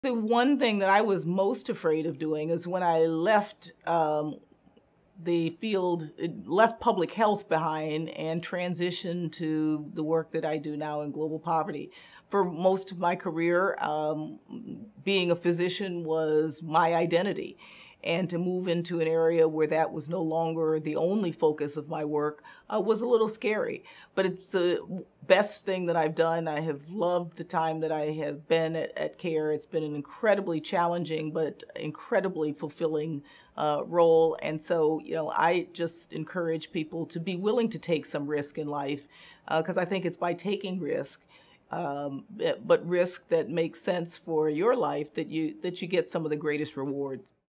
Nonprofit CEO: Did It Anyway - Dr Helene Gayle Career Girls Role Model
Watch Dr. Helene Gayle, CEO of CARE, share valuable career guidance and life advice with girls. Learn how to become an nonprofit CEO and watch her full interview